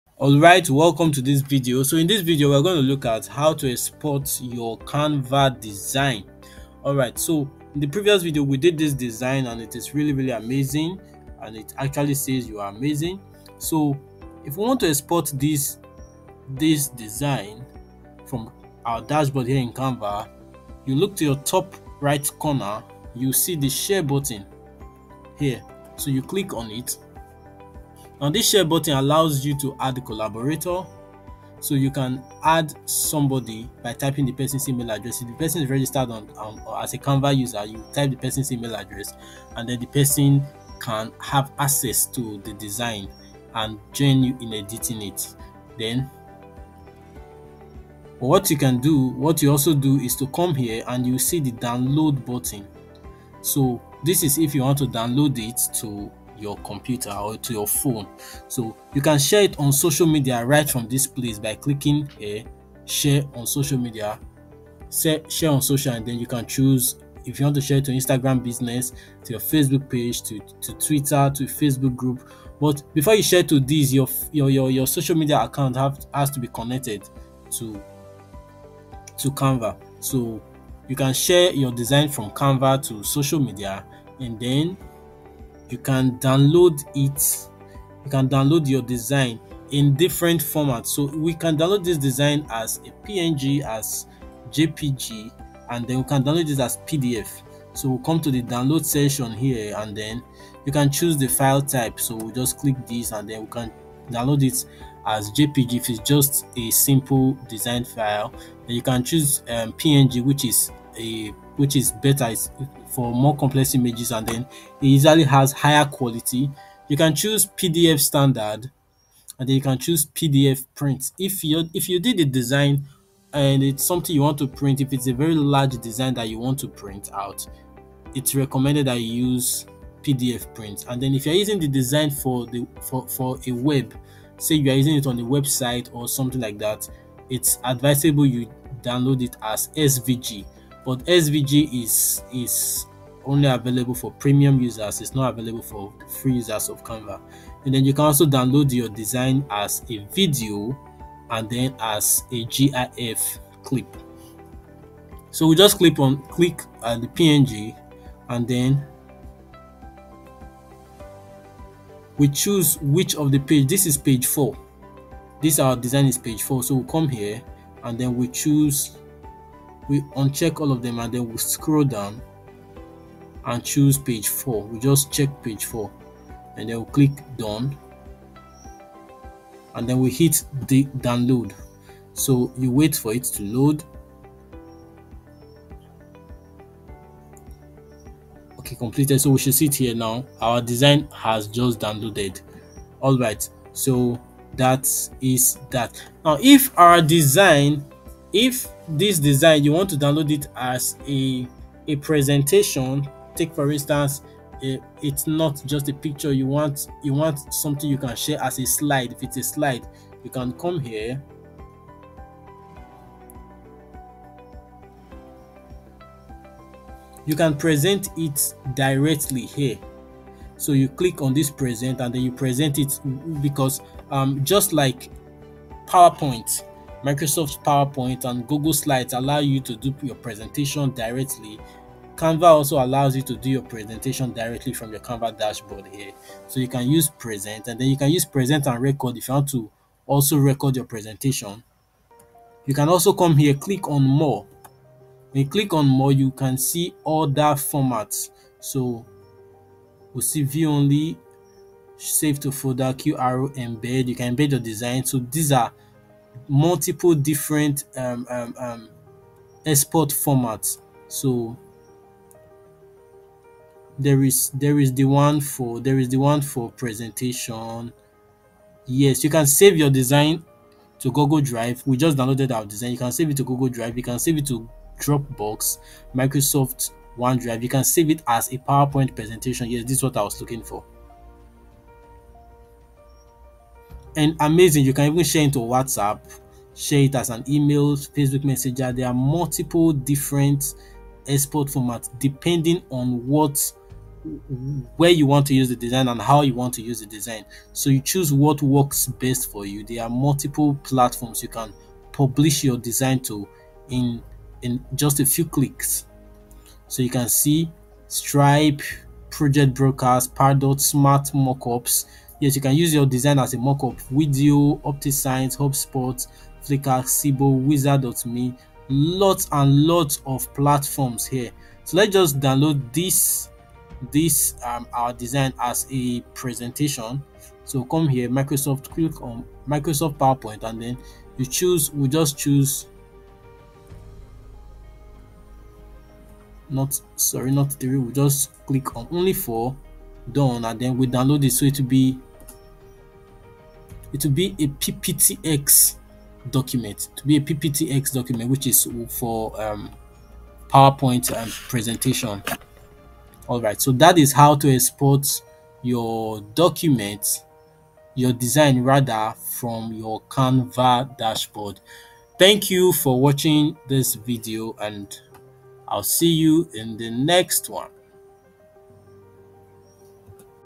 How to export your Canva Design
In this video, I describe the different formats in which you can export your canvas design and for the different purposes.